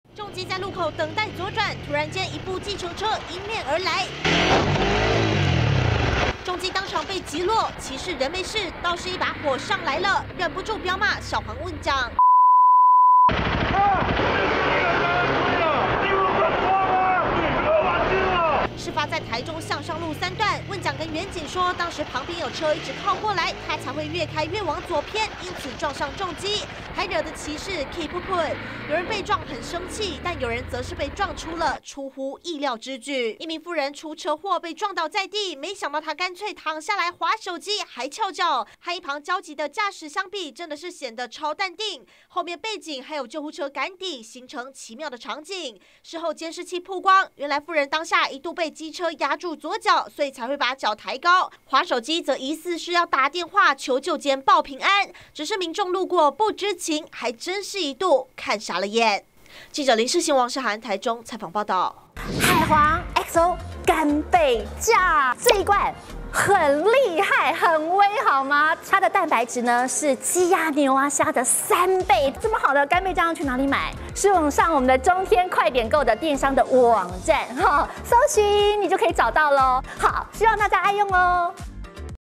【每日必看】小黃擊落重機"被飆罵" 婦遭撞倒竟"翹腳滑手機"20230104 @中天新聞CtiNews​
有人被撞很生氣，有人被撞則是超淡定!台中向上路，有重機直行，遭逆向計程車撞上，騎士氣得在大街上狂飆罵運將，而台中西屯區二段，有名婦人騎車遭休旅車，撞噴倒地，沒想到，婦人索性直接原地躺著，滑起手機來，還翹腳在等救護車，讓路人看傻了眼。